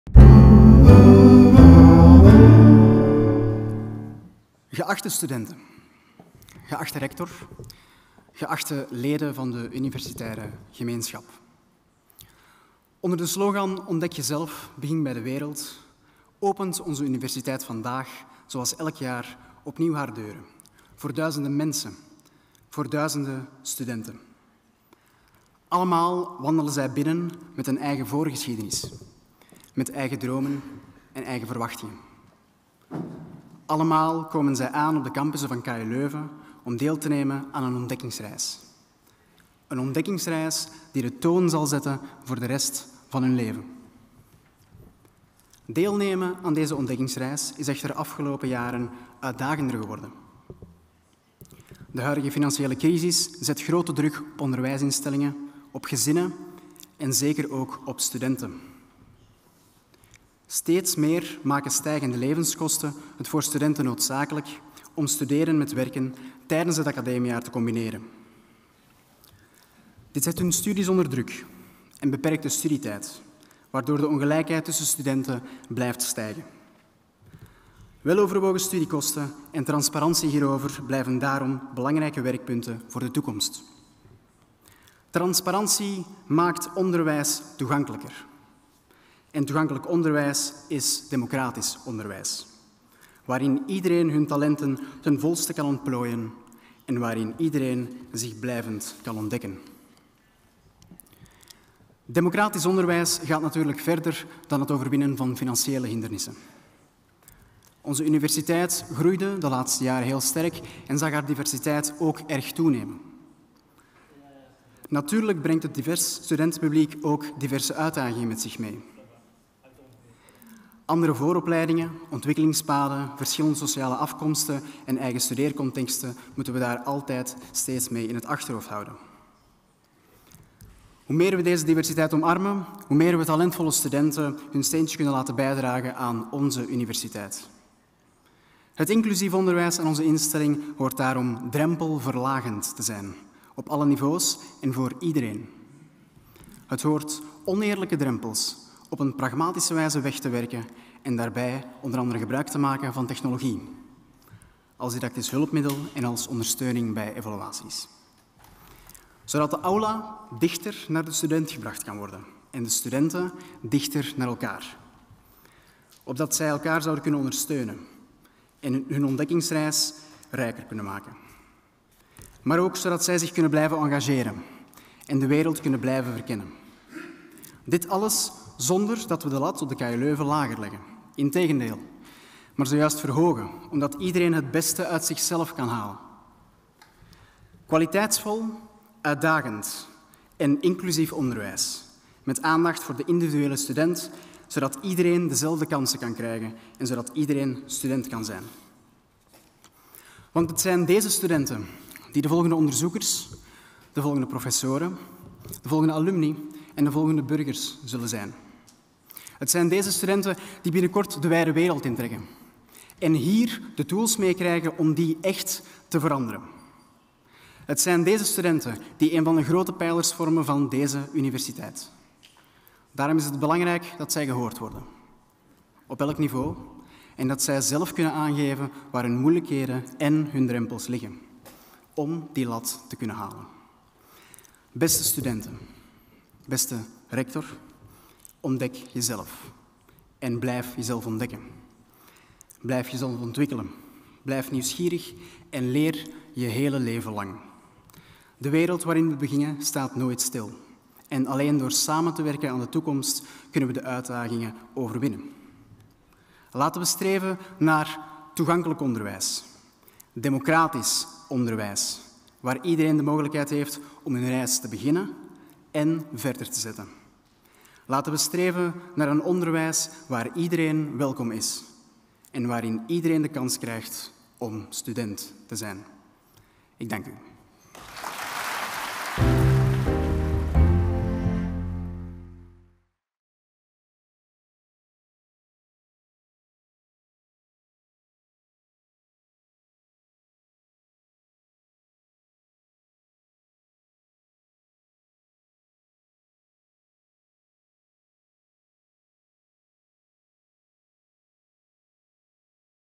Toespraak Ondervoorzitter Studentenraad A. Vandingenen Opening Academiejaar 2023-2024 | KU Leuven
"Ontdek jezelf en blijf jezelf ontdekken."

Duizenden studenten met eigen dromen en eigen verwachtingen beginnen het nieuwe academiejaar. Ondervoorzitter Studentenraad Andreas Vandingenen spreekt over "een ontdekkingsreis die de toon zal zetten voor de rest van hun leven".

Toch stelt hij vast dat de financiële crisis de ongelijkheid tussen studenten doet toenemen.  De studentenraadvoorzitter vraagt daarom meer transparantie over de studiekosten.

Andreas Vandingenen wijst ook op de toenemende diversiteit.  "Hoe meer we deze diversiteit omarmen, hoe meer talentvolle studenten hun steentje kunnen bijdragen aan onze universiteit." Hij pleit er nog voor om oneerlijke drempels weg te werken en tegelijk de lat te verhogen, "omdat iedereen het beste uit zichzelf kan halen".

➡️ Alle toespraken en video's in het kader van de Opening van het Academiejaar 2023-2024 aan KU Leuven